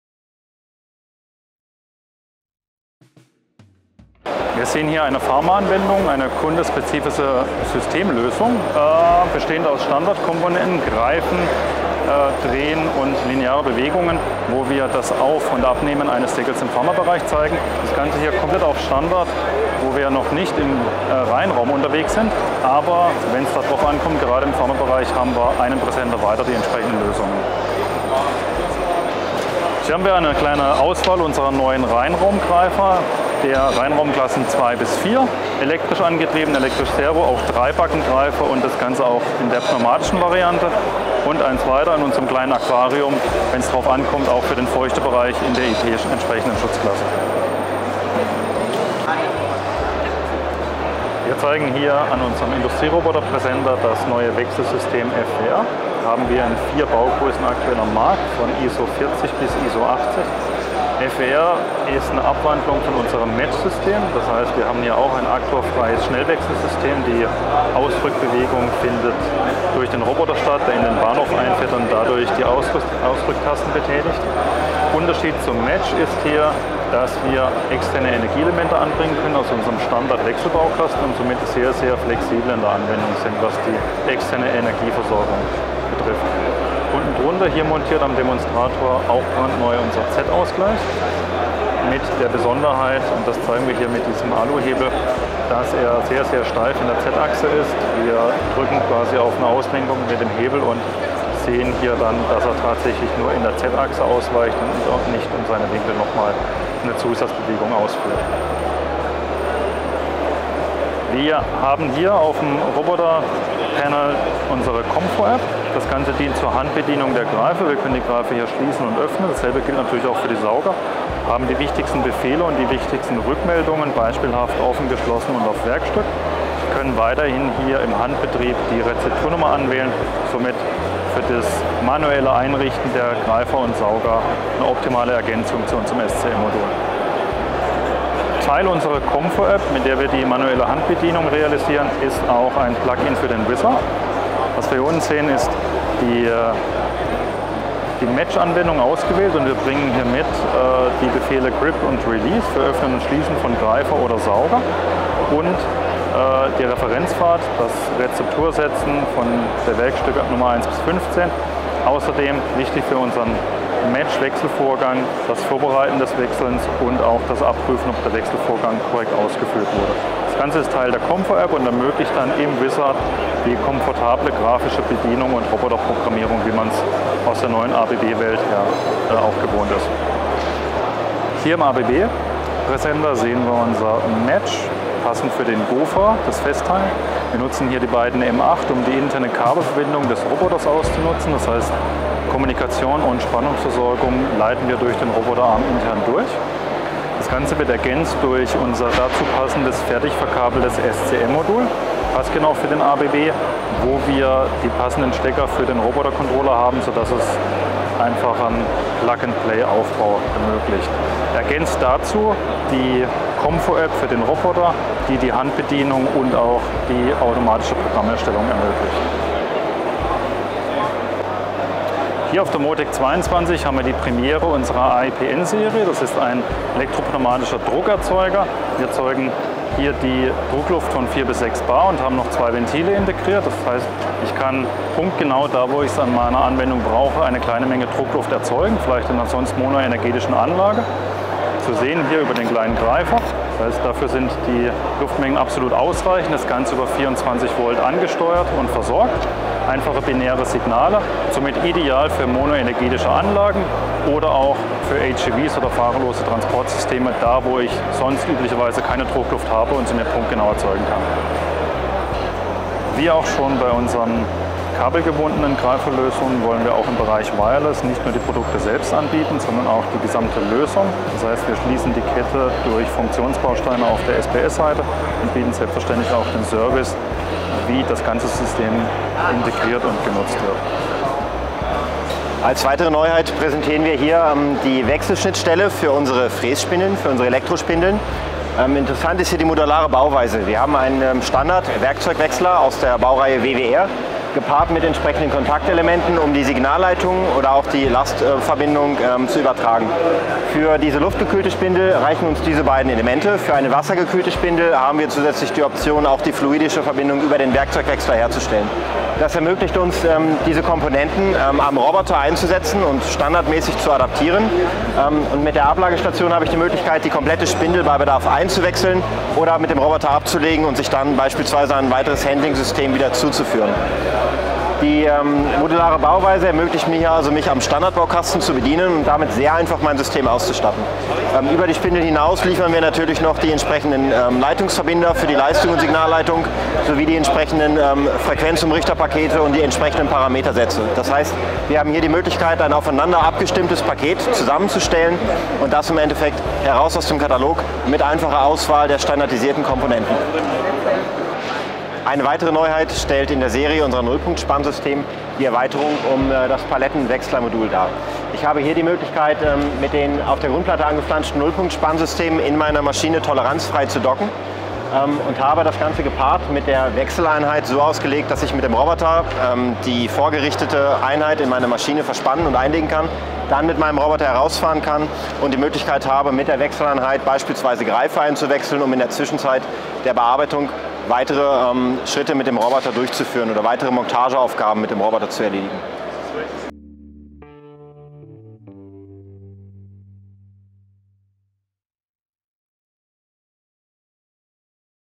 Neuheiten der Zimmer Group auf der Motek
Die Motek 2022 war wieder eine sehr spannende und abwechslungsreiche Möglichkeit, mit Interessenten und Kunden in Kontakt zu treten und interessante Gespräche zu führen. Mit vielen Produktvorstellungen gab es hier am Stand der Zimmer Group so einiges zu sehen und entdecken.

Wir freuen uns aufs nächste Mal!

Finden Sie hier die Termine unserer Messen in 2023 und kommen Sie vorbei: